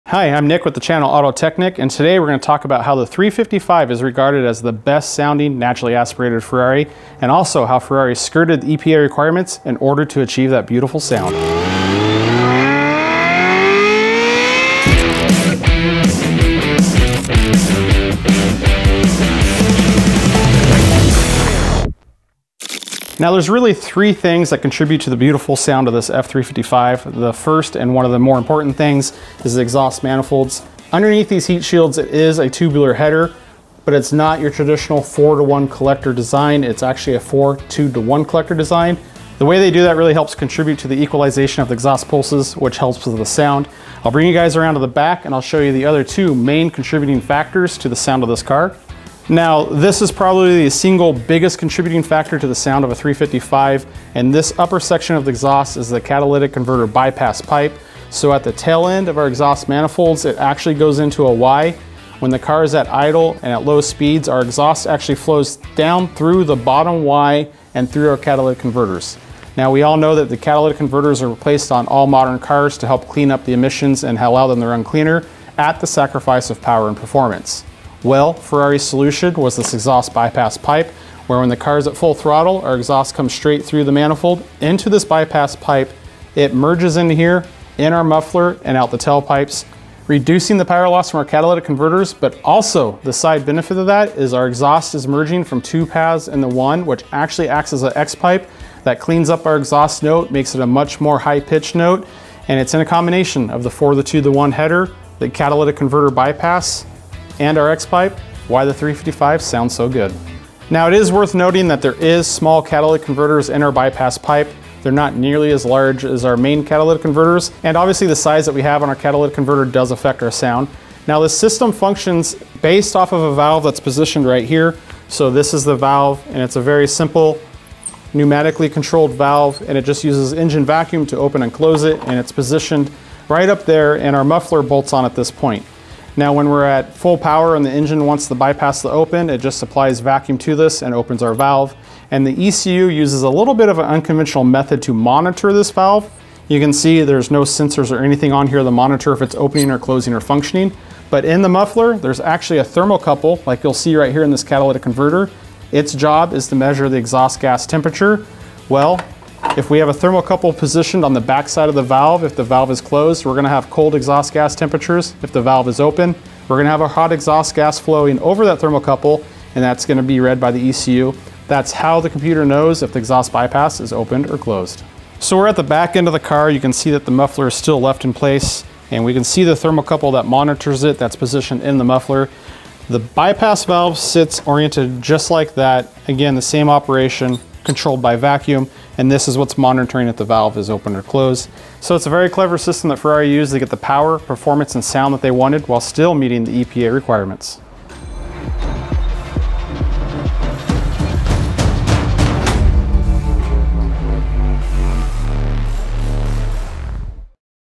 How Ferrari Made the F355 Sound Incredible (and EPA Compliant)
This is how Ferrari's Dry Sump Oil system works on the F355.

WyoTech's video team met up with Nicolas Martinson from AutoTechNick, where he showed us some interesting things about a Ferrari F355 he is working on. Nick is a WyoTech alumni and a Ferrari Master Technician.

Ferrari engineering sets itself apart in the F355 with the dry sump oiling system, which is commonly found in high performance and acceleration vehicles.

Want to follow in his footsteps?